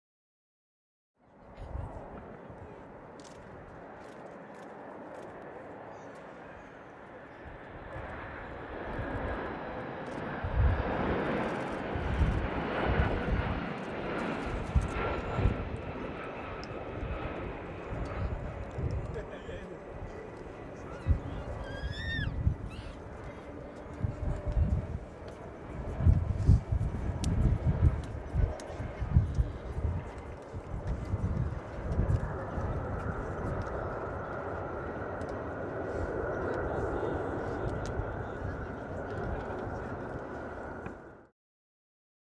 [13/Mar/2005 - Kansai] EG742_JA8130_KIX050313
JAA Boeing 747-246B at Kansai Airport 13/Mar/2005
Camera: Canon XL2